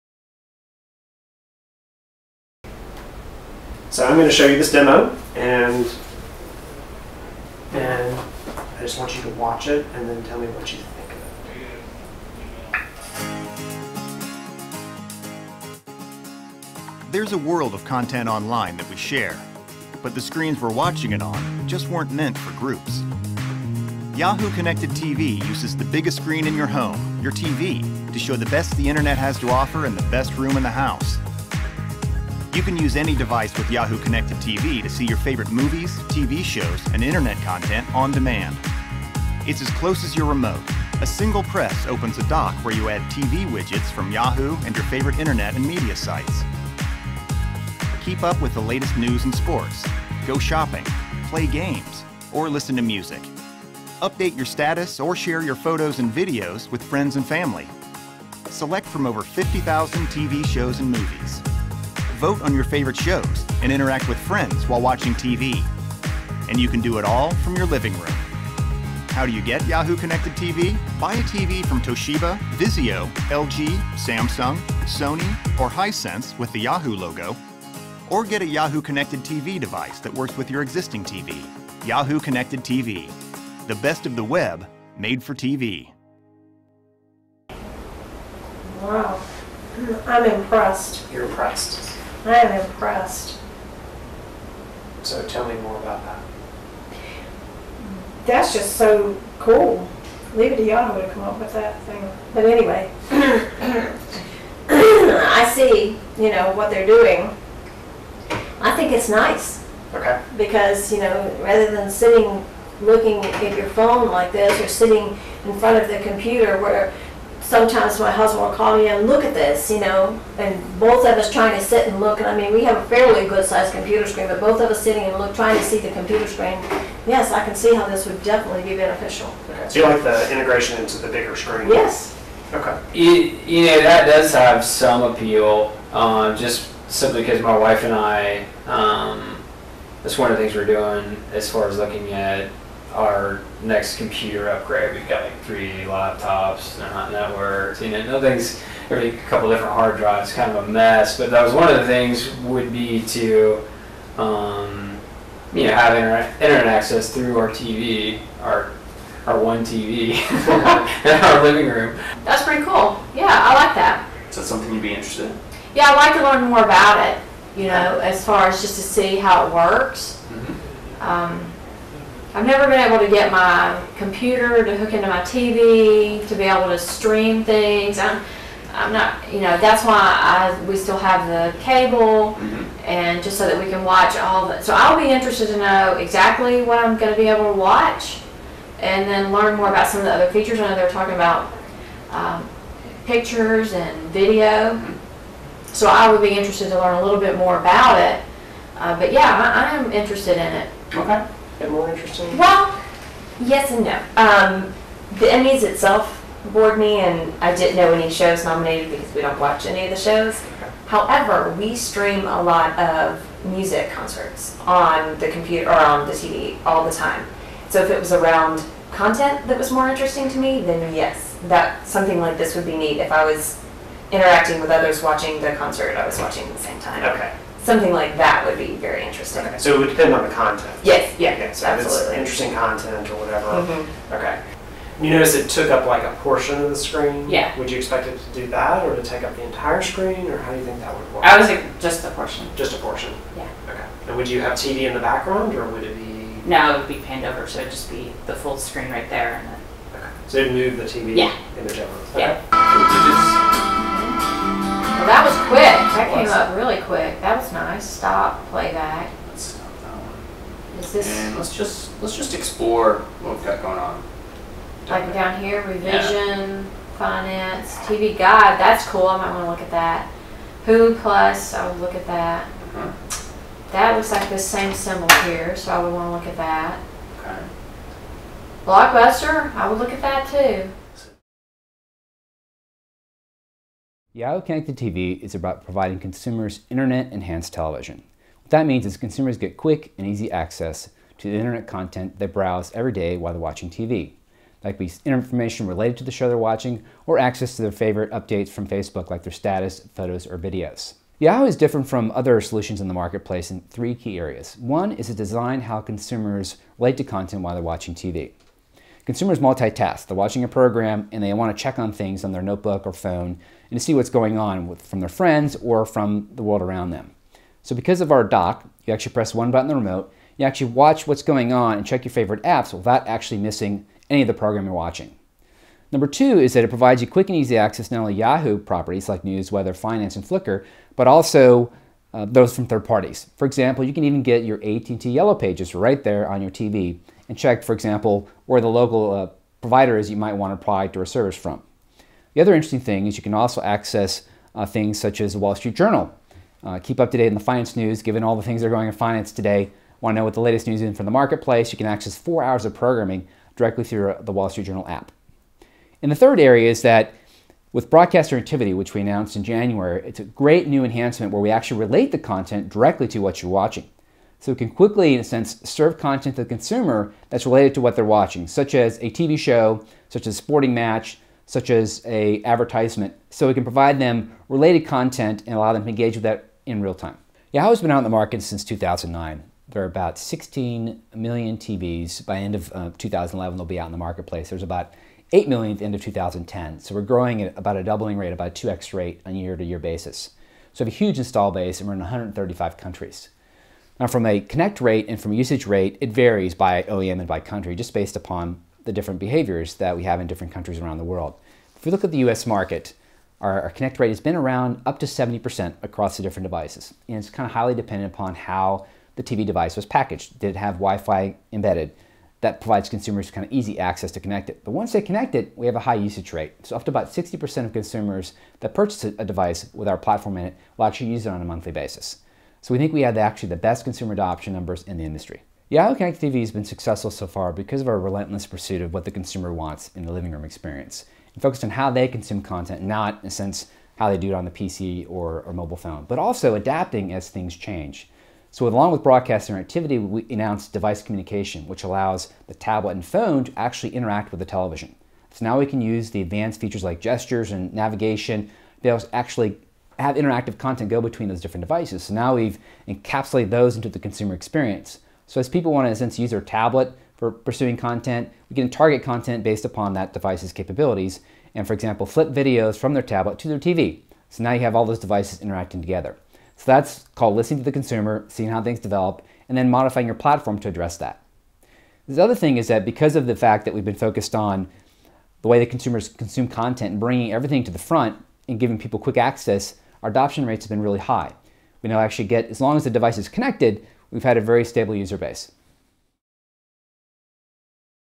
Yahoo! Connected TV Video Short
Learn more about Yahoo!'s Connected TV Product and what users have to say about it! Also, hear from Russ Schafer, Sr. Director of Global Consumer and Business Marketing for Yahoo! Connected TV.